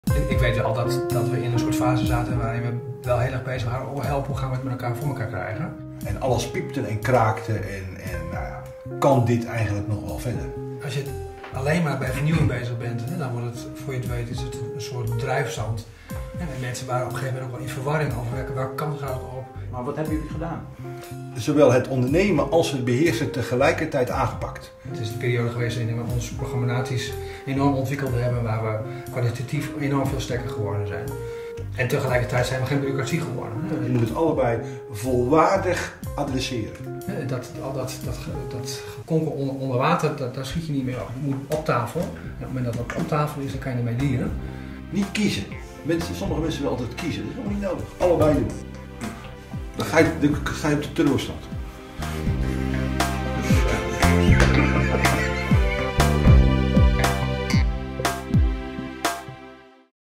Way of working ombuigen? Paradoxen! [Trailer]
Directeur Andries Schuttinga en voormalig directeur Leo Visser vertellen hoe ze de way of working van Red een Kind hebben omgebogen. Dit is een preview (trailer).

Red een Kind heeft een cruciale transformatie doorgemaakt. Hierin werd de paradox ondernemerschap-beheersing ingezet waardoor Red een Kind in de markt bleef.

Dit project werd begeleid door Frank Ottenhoff